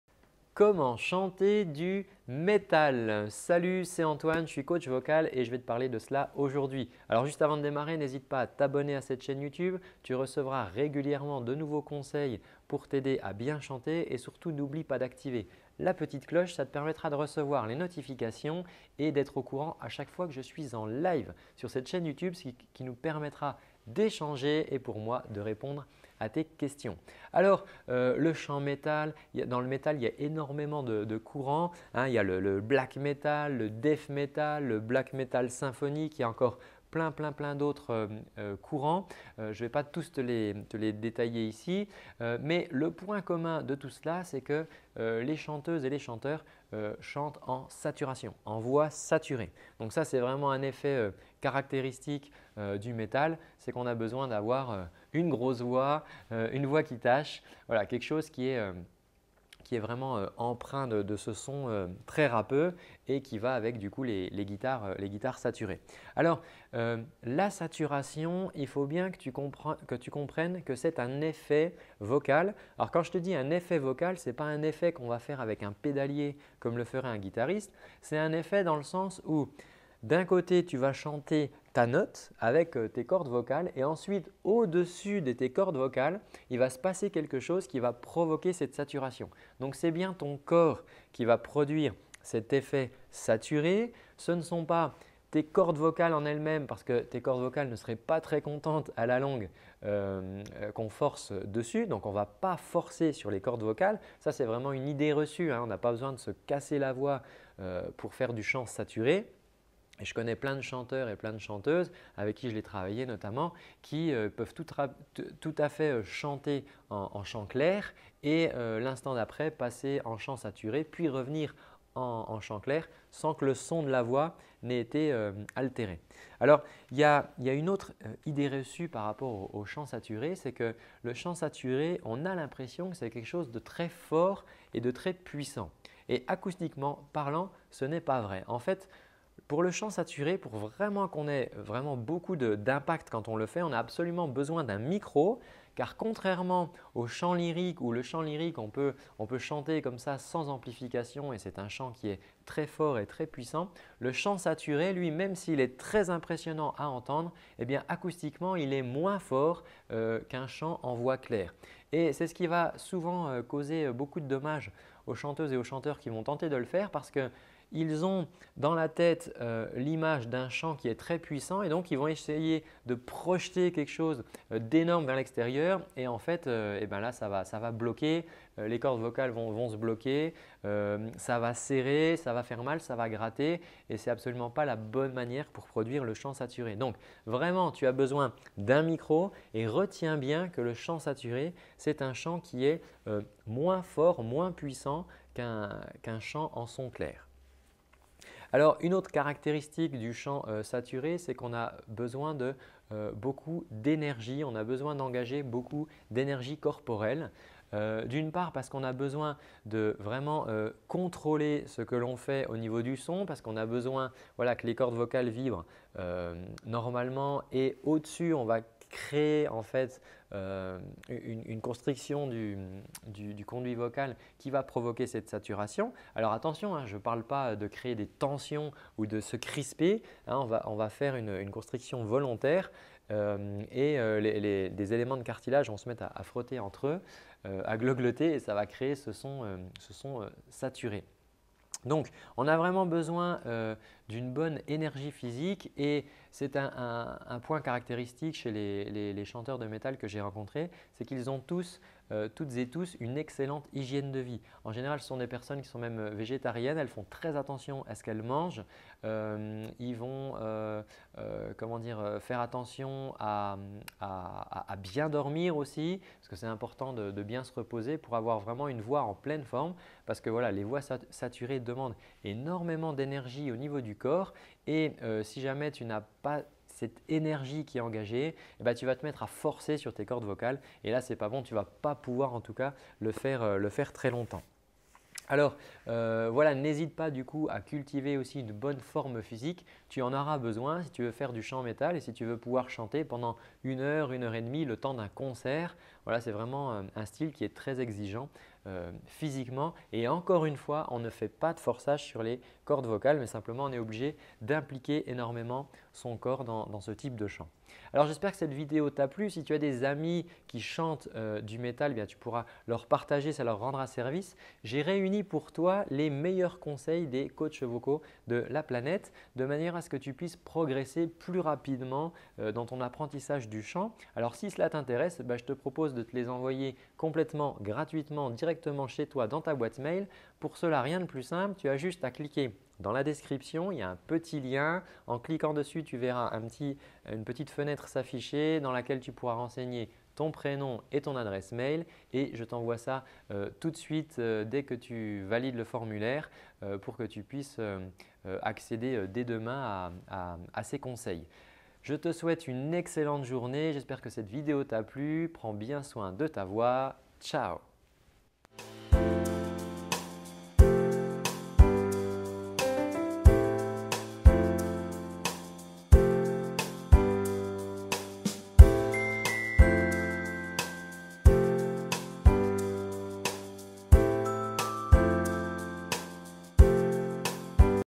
Comment chanter du metal ?
Alors le chant métal, dans le métal, il y a énormément de courant. Il y a le black metal, le death metal et le black metal symphonique. Il y a encore plein d’autres courants. Je ne vais pas tous te les détailler ici. Mais le point commun de tout cela, c’est que les chanteuses et les chanteurs chantent en saturation, en voix saturée. Donc cela, c’est vraiment un effet caractéristique du métal, c’est qu’on a besoin d’avoir une grosse voix, une voix qui tâche, quelque chose qui est vraiment empreint de ce son très râpeux et qui va avec du coup les guitares saturées.

Alors la saturation, il faut bien que tu comprennes que c’est un effet vocal. Alors, quand je te dis un effet vocal, ce n’est pas un effet qu’on va faire avec un pédalier, comme le ferait un guitariste. C’est un effet dans le sens où d’un côté tu vas chanter ta note avec tes cordes vocales ensuite au-dessus de tes cordes vocales, il va se passer quelque chose qui va provoquer cette saturation. C’est bien ton corps qui va produire cet effet saturé. Ce ne sont pas tes cordes vocales en elles-mêmes parce que tes cordes vocales ne seraient pas très contentes à la longue qu’on force dessus.

On ne va pas forcer sur les cordes vocales. C’est vraiment une idée reçue. On n’a pas besoin de se casser la voix pour faire du chant saturé.

Alors, il y a une autre idée reçue par rapport au chant saturé, c’est que le chant saturé, on a l’impression que c’est quelque chose de très fort et de très puissant. Acoustiquement parlant, ce n’est pas vrai. En fait pour le chant saturé pour vraiment qu’on ait beaucoup d’impact quand on le fait, on a absolument besoin d’un micro. Car contrairement au chant lyrique où le chant lyrique on peut chanter comme cela sans amplification et c’est un chant qui est très fort et très puissant, le chant saturé, lui-même, s’il est très impressionnant à entendre, acoustiquement, il est moins fort qu’un chant en voix claire.

C’est ce qui va souvent causer beaucoup de dommages aux chanteuses et aux chanteurs qui vont tenter de le faire parce qu’ils ont dans la tête l’image d’un chant qui est très puissant. Ils vont donc essayer de projeter quelque chose d’énorme vers l’extérieur. Et en fait là, cela va bloquer. Les cordes vocales vont se bloquer, cela va serrer, cela va faire mal, cela va gratter et ce n’est absolument pas la bonne manière pour produire le chant saturé. Donc vraiment, tu as besoin d’un micro et retiens bien que le chant saturé, c’est un chant qui est moins fort, moins puissant qu’un chant en son clair.

Alors une autre caractéristique du chant saturé, c’est qu’on a besoin de beaucoup d’énergies. On a besoin d’engager beaucoup d’énergies corporelles. D’une part parce qu’on a besoin de vraiment contrôler ce que l’on fait au niveau du son, parce qu’on a besoin que les cordes vocales vibrent normalement et au-dessus on va créer en fait une constriction du conduit vocal qui va provoquer cette saturation. Alors attention, je ne parle pas de créer des tensions ou de se crisper.

Donc, on a vraiment besoin d’une bonne énergie physique et c’est un point caractéristique chez les chanteurs de métal que j’ai rencontrés, c’est qu’ils ont toutes et tous une excellente hygiène de vie.

Alors, n’hésite pas du coup à cultiver aussi de bonne forme physique, tu en auras besoin si tu veux faire du chant métal et si tu veux pouvoir chanter pendant une heure, une heure et demie, le temps d’un concert. C’est vraiment un style qui est très exigeant physiquement.

Le résultat ?

Antoine